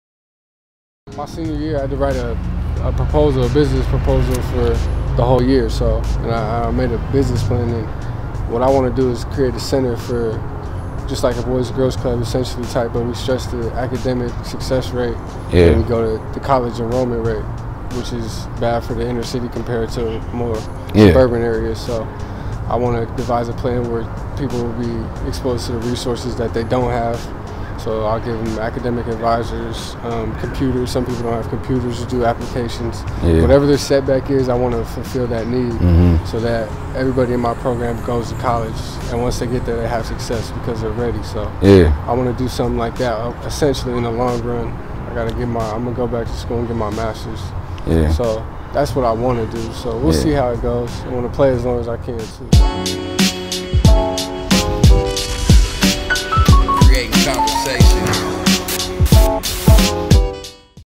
Davonte Lacy: Business Plan (CC Shorts)
Creating Conversation Season 1
Episode 2: Lacy for 3
Featuring: DaVonté Lacy, Making Music Clothing, Isaiah Jimenez, DJ Taber, Simone Bullinger, Candice Holder, Dazjon Johnson, Evan Cook, & Natasha Trinidad

Host: Isaiah Jimenez
Produced & Directed by: Andre Adams
Photography: Dennis Adjetey
Digital Illustration: LuQuam Thompson
Assistant Camera: Teena Thach & Dennis Adjetey
Executive Producers: Isaiah Jimenez & Andre Adams

Special thanks to everyone involved, the photographers & videographers responsible for stock footage used, & the entire Creating Conversation crew